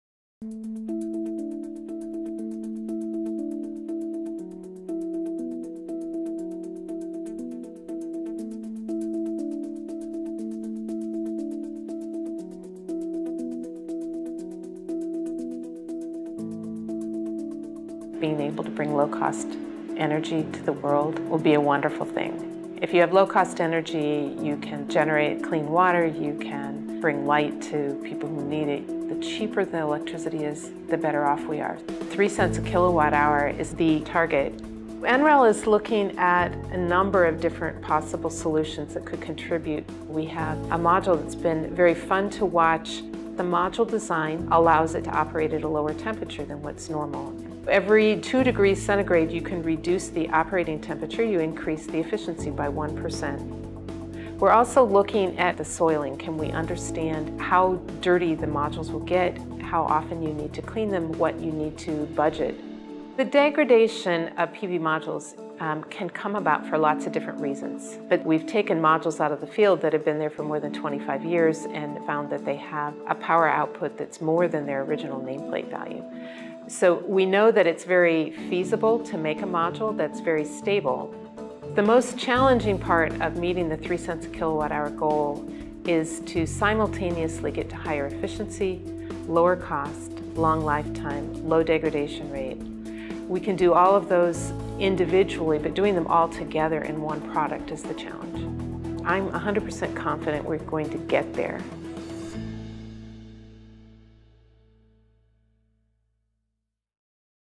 Improving Photovoltaic Technology to Lower Solar Costs
As the SunShot Initiative begins to work toward new targets for 2030, projects at the National Renewable Energy Laboratory are already making great progress. Learn more about SunShot’s 2030 goals at energy.gov/sunshot2030.